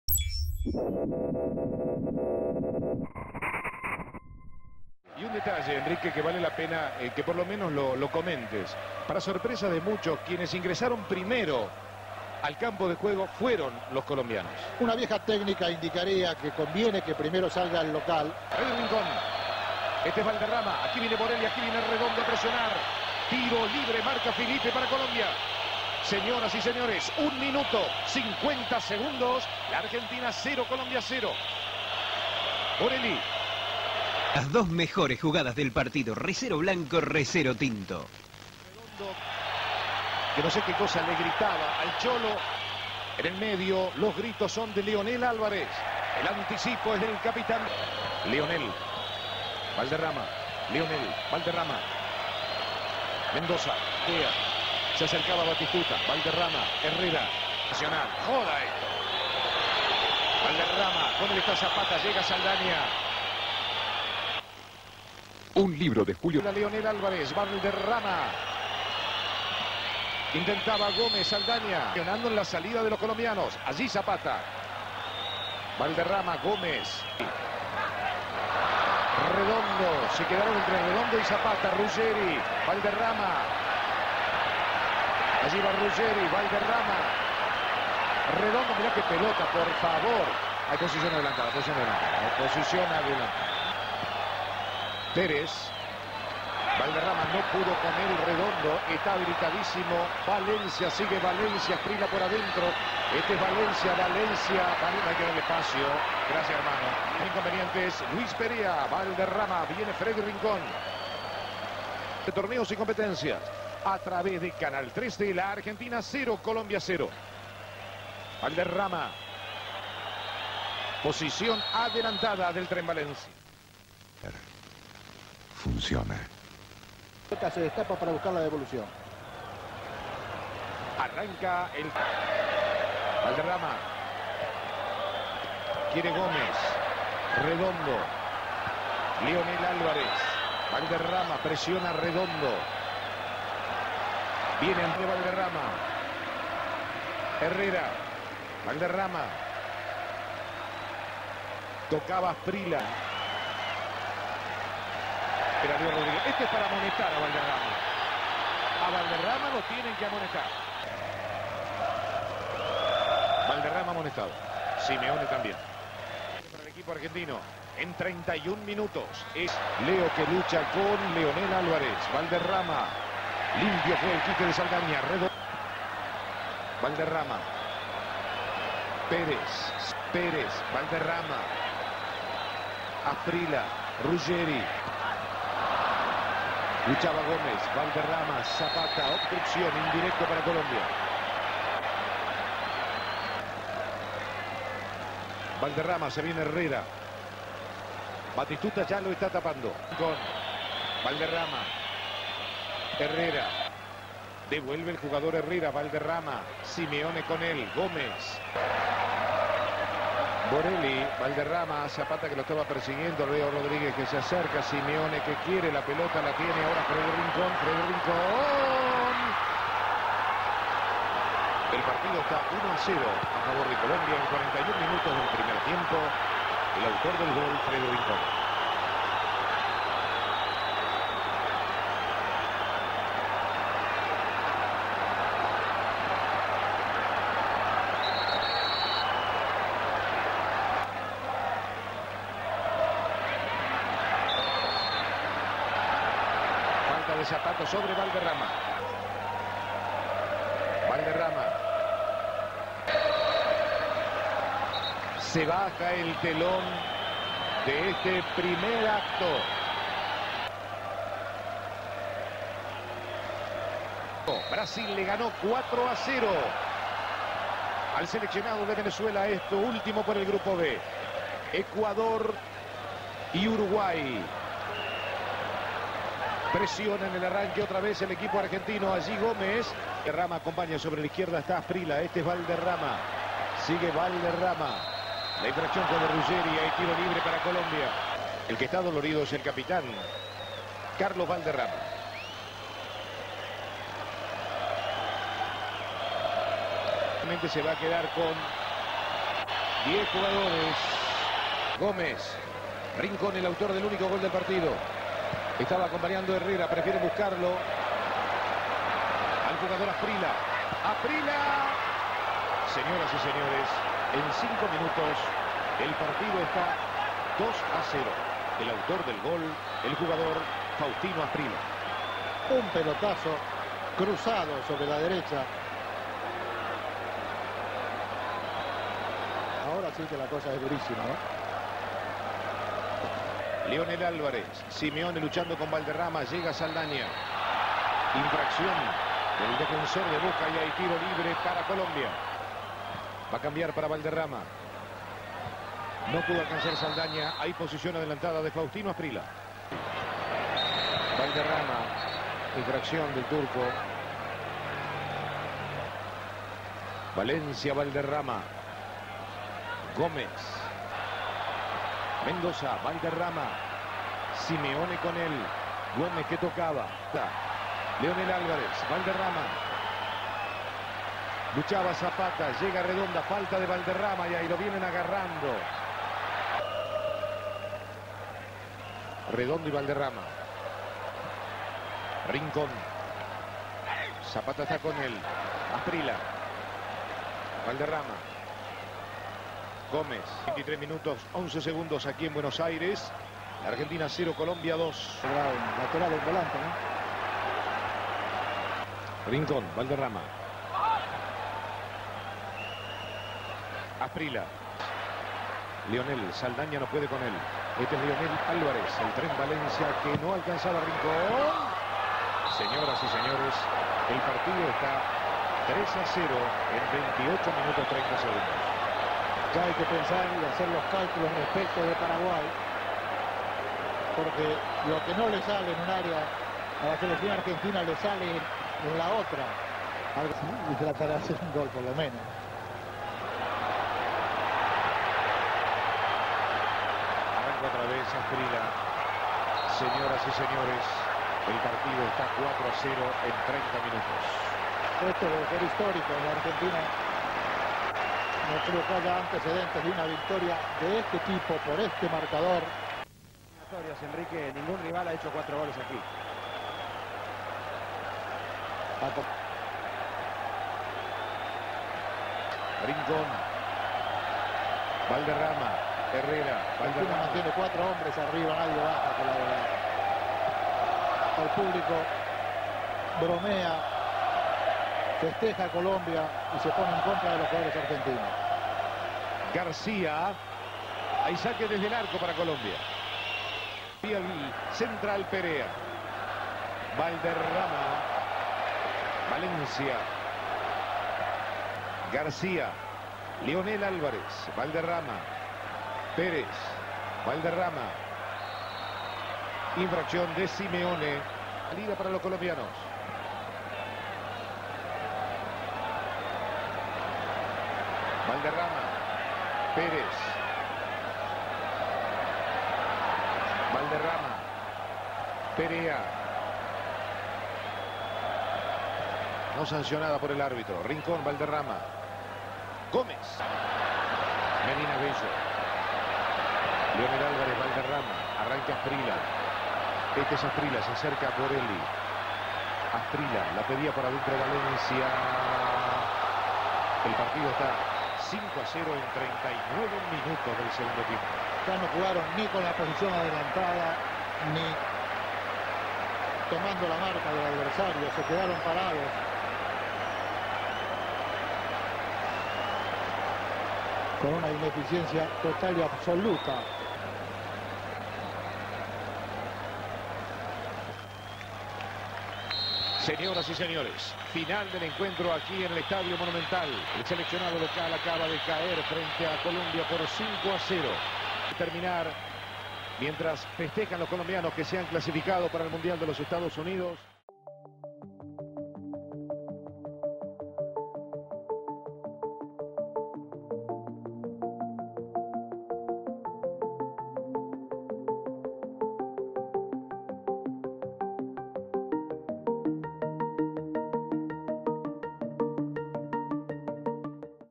Ni Redondo ni Ruggeri pudieron pararlo, actuación legendaria de Valderrama vs Argentina (1993)
Argentina 0-5 Colombia, Victoria historica